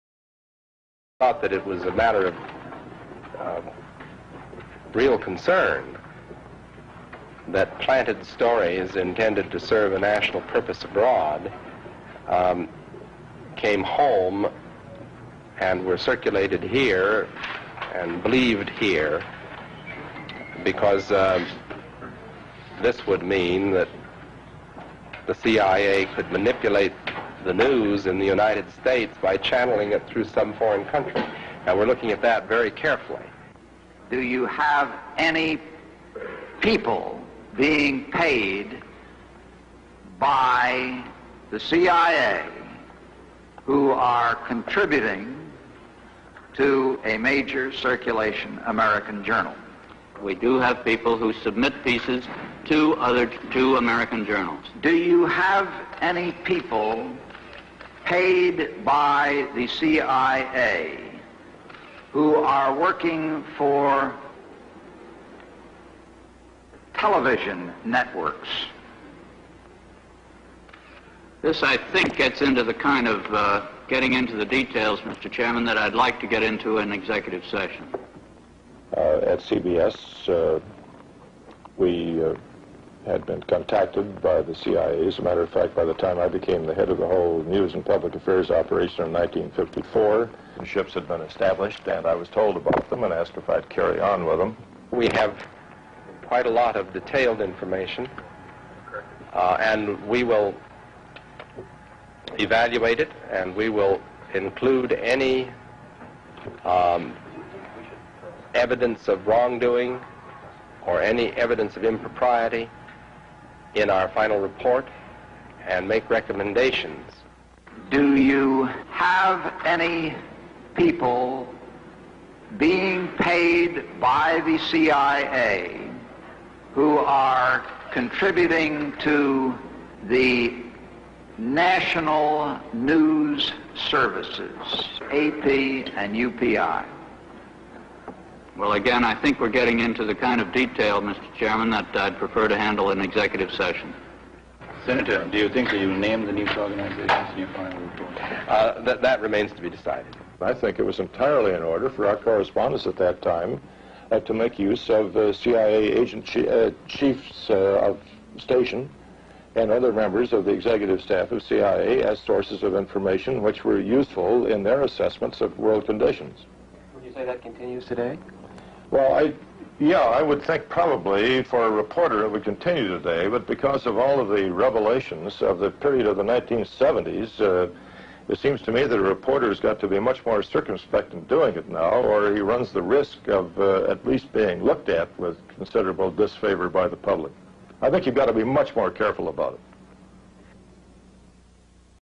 CIA Manipulates America Press (1975 - ____ )
In 1975 and 1976, the Church Committee published fourteen reports on various U.S. intelligence agencies', formation, operations, and the alleged abuses of law and of power that they had committed, with recommendations for reform, some of which were later put in place.

Among the matters investigated were attempts to assassinate foreign leaders, including Patrice Lumumba of the Democratic Republic of the Congo, Rafael Trujillo of the Dominican Republic, the Diem brothers of Vietnam, Gen. René Schneider of Chile and Director of Central Intelligence Allen Welsh Dulles's plan (approved by President Dwight D. Eisenhower) to use the Sicilian Mafia to kill Fidel Castro of Cuba.

Under recommendations and pressure by this committee, President Gerald Ford issued Executive Order 11905 (ultimately replaced in 1981 by President Reagan's Executive Order 12333 expanding CIA powers) to ban U.S. sanctioned assassinations of foreign leaders.

Together, the Church Committee's reports have been said to constitute the most extensive review of intelligence activities ever made available to the public. Much of the contents were classified, but over 50,000 pages were declassified under the President John F. Kennedy Assassination Records Collection Act of 1992.